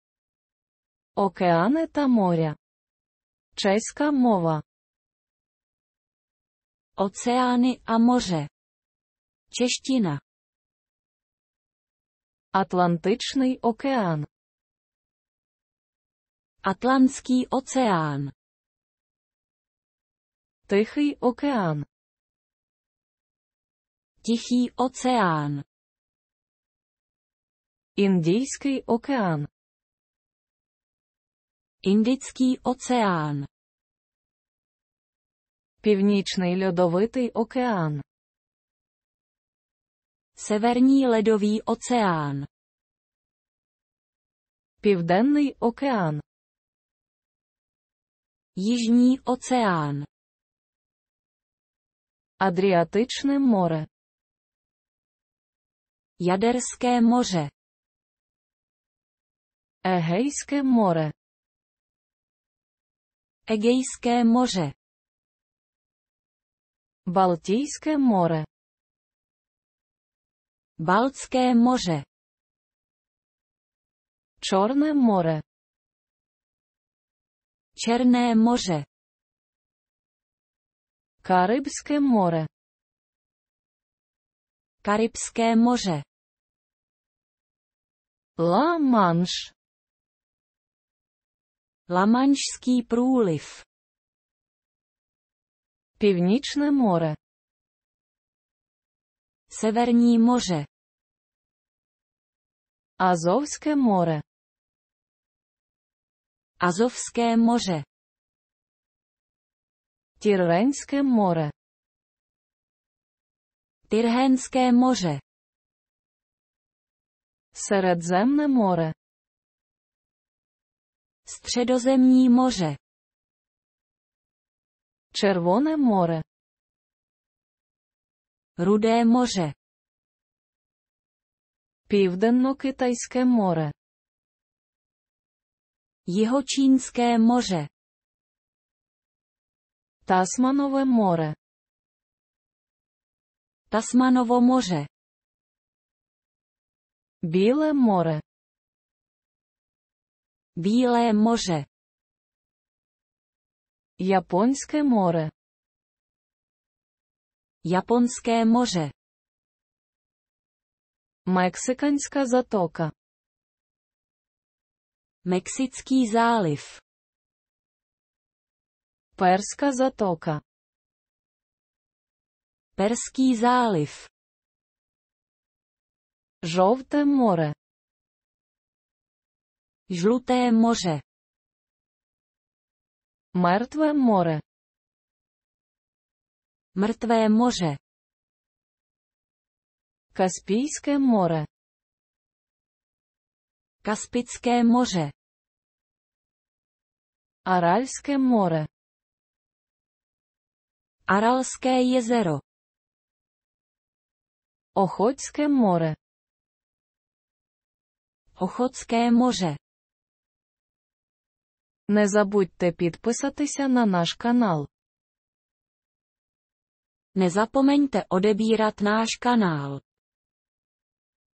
Океани та моря (Чеська мова) (uk-cs)
Fone TV
uk-cs-31
Oceány a moře (Čeština)
Атлантичний океан - Atlantský oceán
Тихий океан - Tichý oceán
Індійський океан - Indický oceán
Північний Льодовитий океан - Severní ledový oceán
Південний океан - Jižní oceán
Адріатичне море - Jaderské moře
Егейське море - Egejské moře
Балтійське море - Baltské moře
Чорне море - Černé moře
Карибське море - Karibské moře
Ла-Манш - Lamanšský průliv
Північне море - Severní moře
Азовське море - Azovské moře
Тірренське море - Tyrhénské moře
Середземне море - Středozemní moře
Червоне море - Rudé moře
Південнокитайське море - Jihočínské moře
Тасманове море - Tasmanovo moře
Біле море - Bílé moře
Японське море - Japonské moře
Мексиканська затока - Mexický záliv
Перська затока - Perský záliv
Жовте море - Žluté moře
Мертве море - Mrtvé moře
Каспійське море - Kaspické moře
Аральське море - Aralské jezero
Охотське море - Ochotské moře